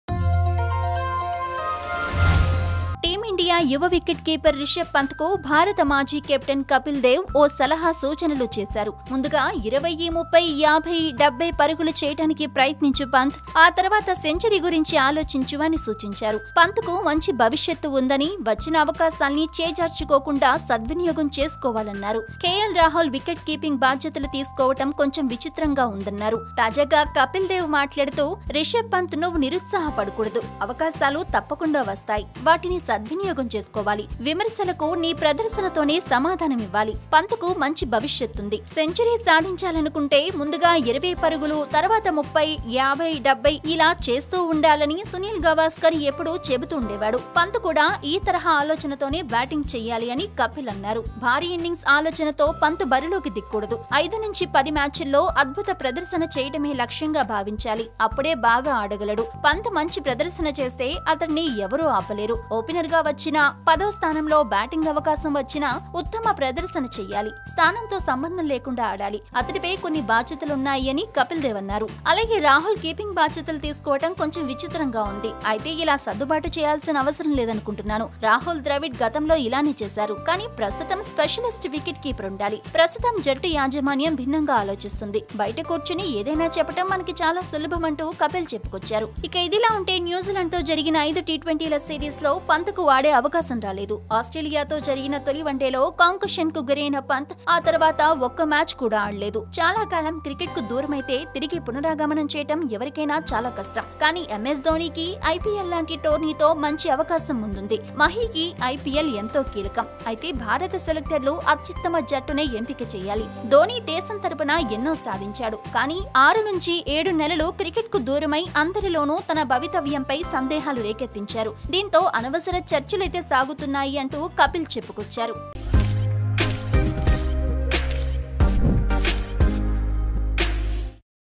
IND VS NZ,1st ODI : Kapil Dev Backs Rishabh Pant To Make A Comeback | Oneindia Telugu
IND VS NZ,1st ODI : One of the major positives for India in the recent past was KL Rahul’s wicket-keeping Work, which gave the team management a chance to add an extra batsman in the lineup. Though the move came out positive, the former skipper Kapil Dev raised his concern over Rishabh Pant’s future in the Indian National Cricket team.
టీమిండియా యువ వికెట్‌కీపర్‌ రిషభ్‌ పంత్‌కు భారత మాజీ కెప్టెన్ కపిల్‌ దేవ్ ఓ సలహా, సూచనలు ఇచ్చారు. ముందుగా 20, 30, 50, 70 పరుగులు చేయటానికి ప్రయత్నించు.. ఆ తర్వాత సెంచరీ గురించి ఆలోచించు అని సూచించారు. పంత్‌కు మంచి భవిష్యత్తు ఉందని, వచ్చిన అవకాశాల్ని చేజార్చుకోకుండా సద్వినియోగం చేసుకోవాలన్నారు. కేఎల్‌ రాహుల్ వికెట్‌ కీపింగ్‌ బాధ్యతలు తీసుకోవడం కొంచెం విచిత్రంగా ఉందన్నారు.
తాజాగా కపిల్‌ దేవ్ మాట్లాడుతూ... 'రిషబ్ పంత్‌ నిరుత్సాహపడకూడదు. అవకాశాలు తప్పకుండా వస్తాయి. వాటిని సద్వినియోగం చేసుకోవాలి. విమర్శకులకు తన ప్రదర్శనతోనే సమాధానం ఇవ్వాలి. పంత్‌కు మంచి భవిష్యత్తు ఉంది. సెంచరీ సాధించాలనుకుంటే ముందుగా 20 పరుగులు చేయటానికి ప్రయత్నించు.. ఆ తర్వాత 20, 30, 50, 70 చేస్తూ సెంచరీ సాధించు అని సునిల్ గావస్కర్‌ చెబుతుండేవాడు. పంత్‌ కూడా ఈ తరహా ఆలోచనతోనే బ్యాటింగ్‌ చేయాలి' అని అన్నారు.
'భారీ ఇన్నింగ్స్‌ ఆలోచనతో పంత్‌ బరిలోకి దిగకూడదు. 5-10 మ్యాచ్‌ల్లో అద్భుత ప్రదర్శన చేయడమే లక్ష్యంగా భావించాలి. అప్పుడే బాగా ఆడగలడు. పంత్‌ మంచి ప్రదర్శన చేస్తే.. అతడిని ఎవరూ ఆపలేరు. ఓపెనర్‌గా వచ్చినా, పదో స్థానంలో బ్యాటింగ్‌ అవకాశం వచ్చినా ఉత్తమ ప్రదర్శన చేయాలి. స్థానంతో సంబంధం లేకుండా ఆడాలి. అతడిపై కొన్ని బాధ్యతలు ఉన్నాయి' అని కపిల్‌ దేవ్ పేర్కొన్నారు.